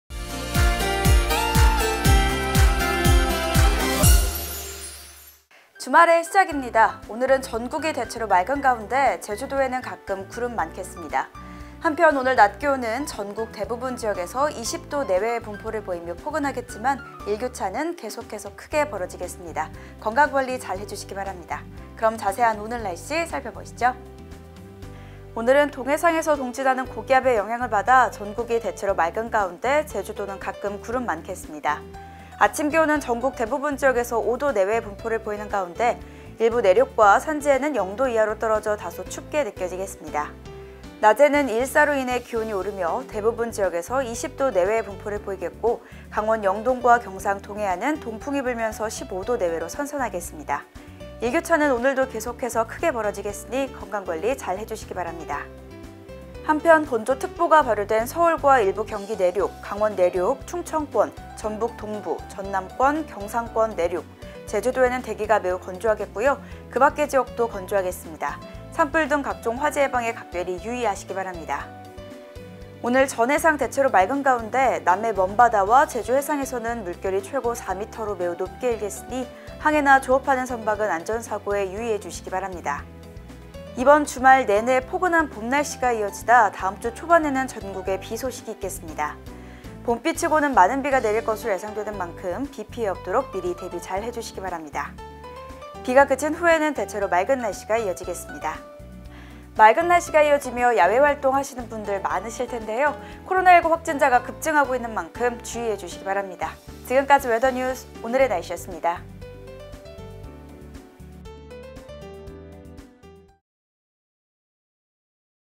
[웨더뉴스] 오늘의 날씨 (4월 10일 7시 기준)
주말의 시작입니다!

오늘은 전국이 대체로 맑은 가운데 제주도에는 가끔 구름 많겠습니다.

한편, 오늘 낮 기온은 전국 대부분 지역에서 20도 내외의 분포를 보이며 포근하겠으나 일교차는 계속해서 크게 벌어지겠으니 건강관리 잘하시기 바랍니다.

그럼 자세한 날씨는 영상으로 함께 알아볼까요~?

오늘의 캐스터 : 김지수